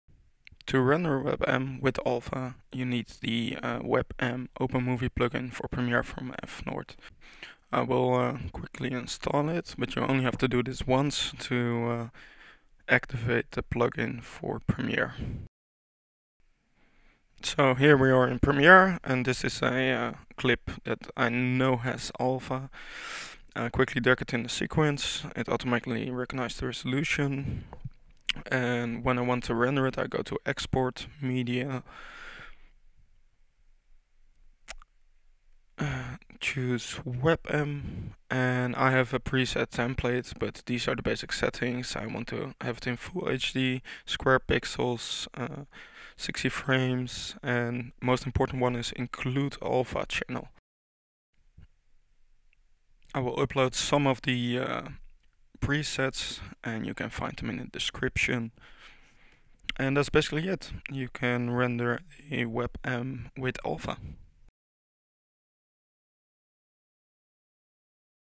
Export WebM with Alpha in Premiere Pro - Short Tutorial
Exporting a WebM clip in Premiere Pro CC with Alpha. This works for Mac and Windows.